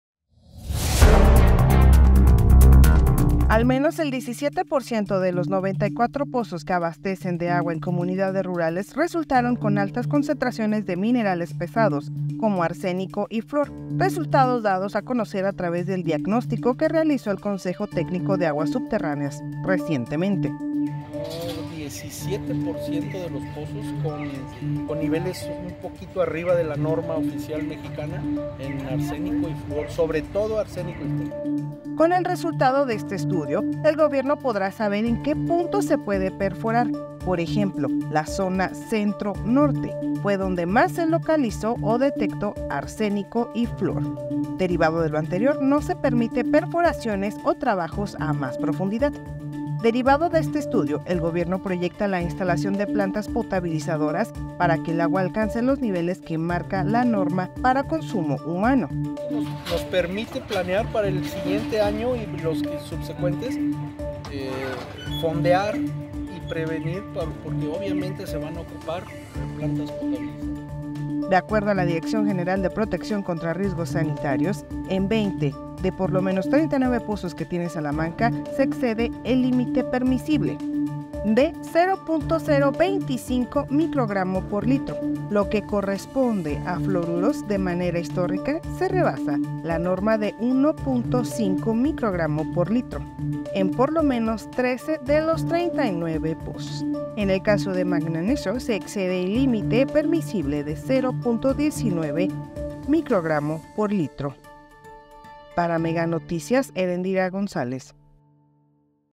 Alerta por Contaminación: Más del 17% de Pozos Rurales Superan Límites de Minerales Peligrosos
ESP AGUA ARSENICO